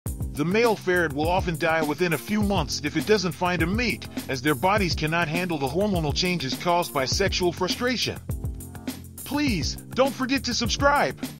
Surprising facts about animals : The ferret
The male ferret will often die within a few months if it doesn't find a mate, as their bodies cannot handle the hormonal changes caused by sexual frustration.